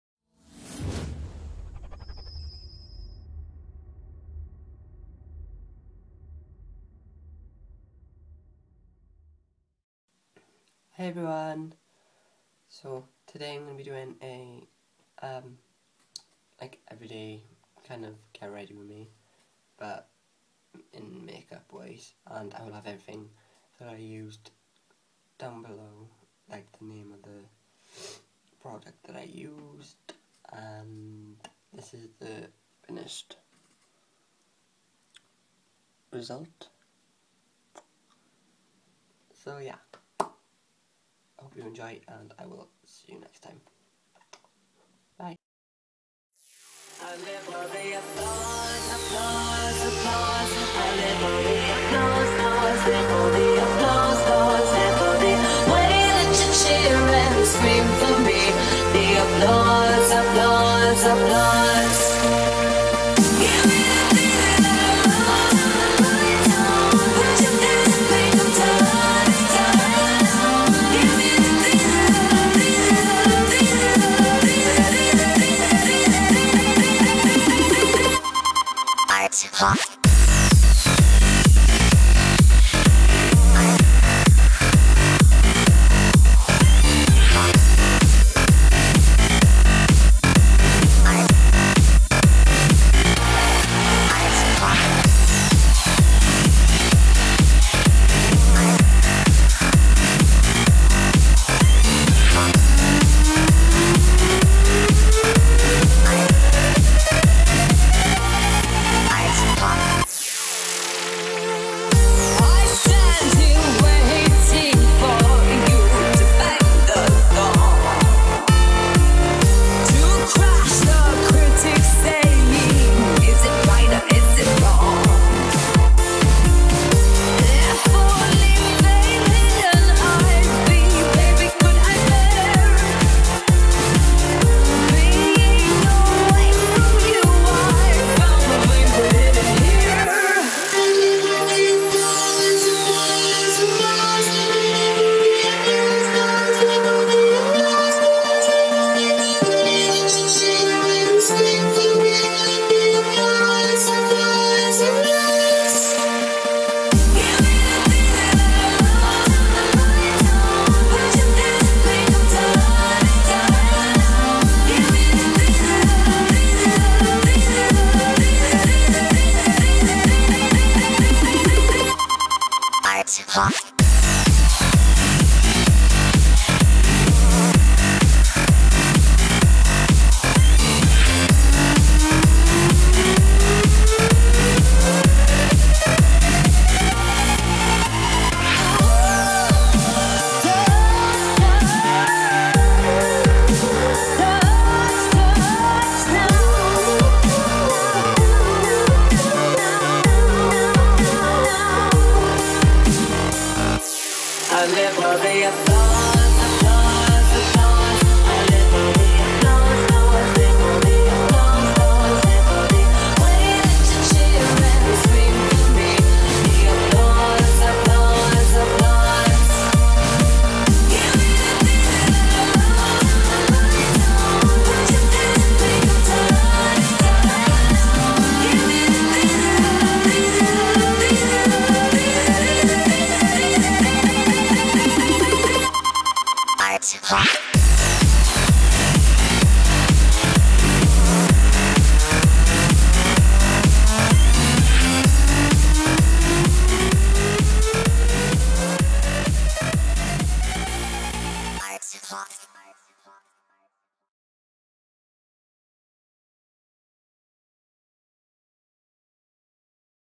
Get Ready With Me #2 | Everyday Makeup
I hope you enjoyed this video! Check out my links & Products below.
WHERE ELSE YOU CAN FIND ME
FYI: All opnions are my own. I'm not being paid for this video!

Products used
NYC PRIMER
REAL TECHNIQUES FACE BRUSH
REAL TECHNIQUES POWDER BRUSH
MUA BLUSH - BUBBLE GUM
RIMEAL LONDON SEXY CURVES MASCARA
MUA STICK CONCEALER - FAIR
W7 PHOTO READY FOUNDATION
MUA PRESSED POWDER SHADE 1 & 4
WET N WILD LIPSTICK - CANDYFLOSS